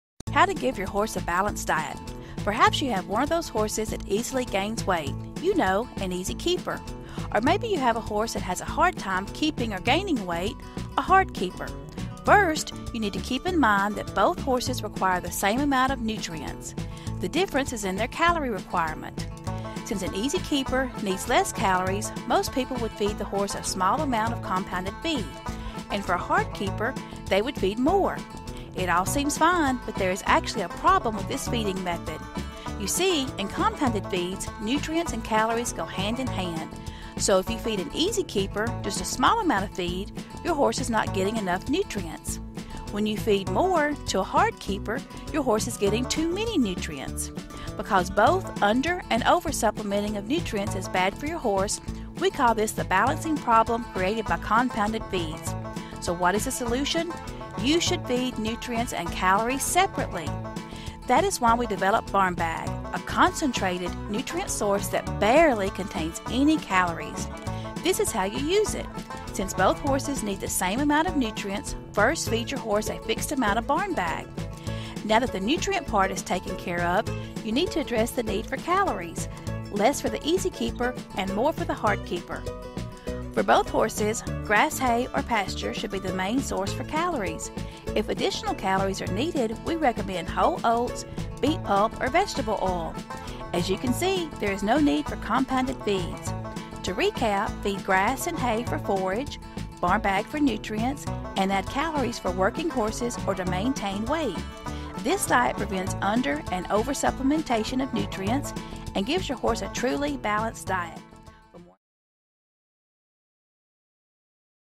How to Give Your Horse a Balanced Diet with Barn Bag
The Barn Bag Pleasure and Performance Horse Pelleted Feed Concentrate is designed to balance hay and pasture diets. Nutrients required for activity and performance are supplied in the correct amounts by the Barn Bag without creating the nervousness and obesity problems often associated with poor quality diets and over-supplementation. Give hay and/or pasture for roughage, Barn Bag for nutrients, and if necessary, whole oats to maintain body weight.